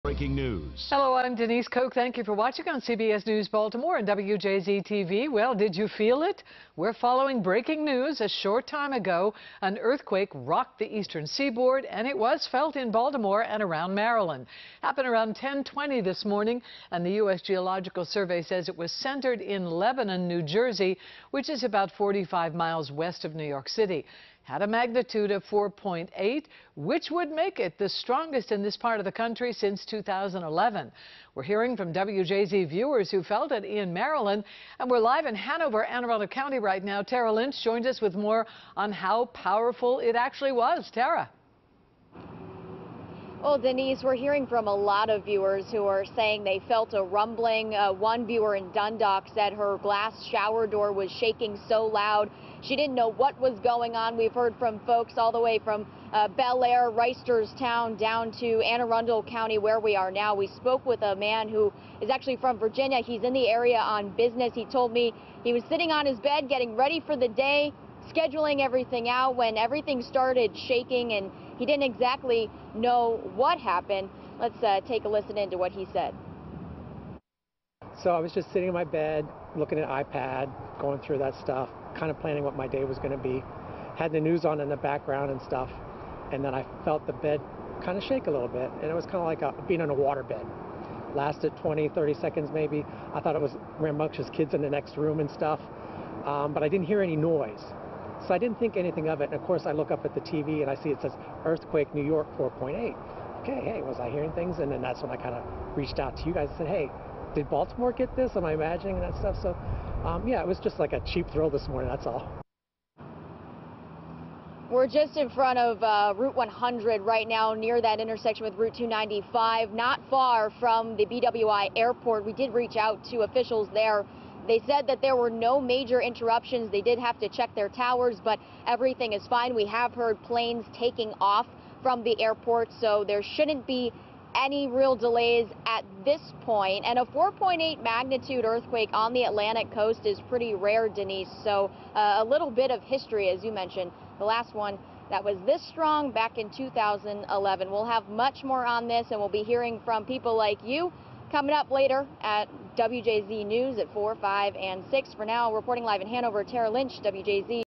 4.8 magnitude earthquake shakes Maryland
The 4.8 magnitude earthquake hit around 10:20 a.m., according to preliminary data from the United States Geological Survey.